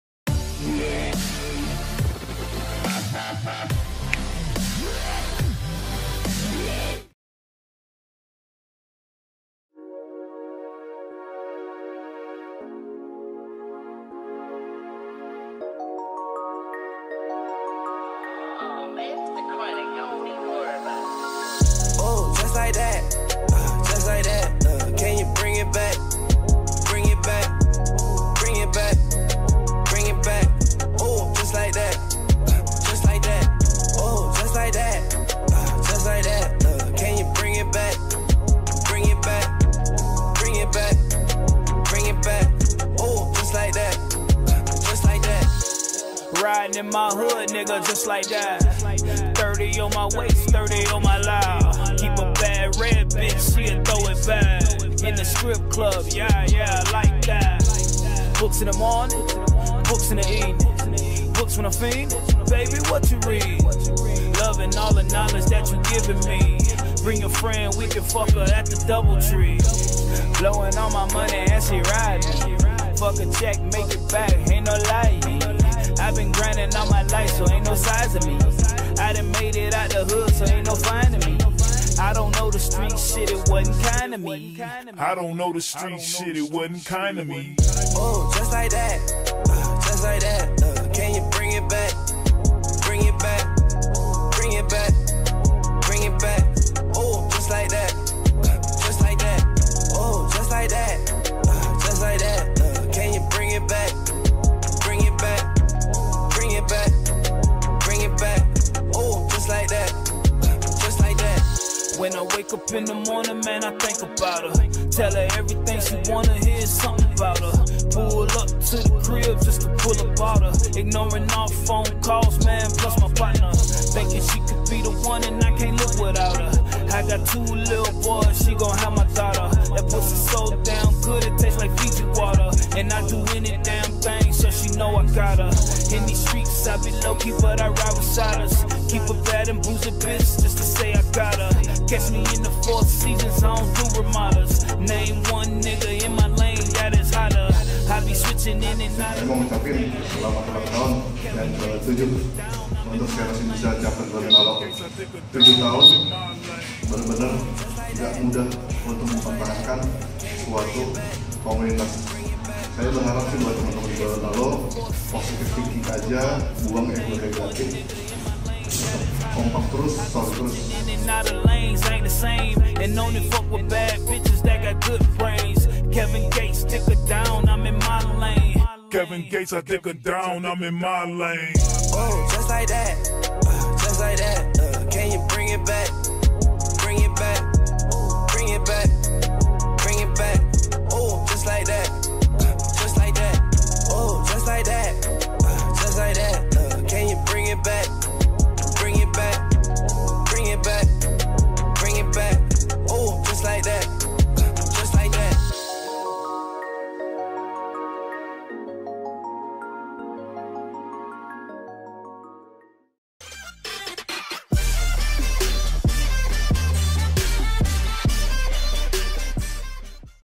UNITY : 7th ANNIVERSARY XEONERS CHAPTER GORONTALO
Lap, buladu, Juli 2018, Xeon owners society rayakan ulang tahun ke 7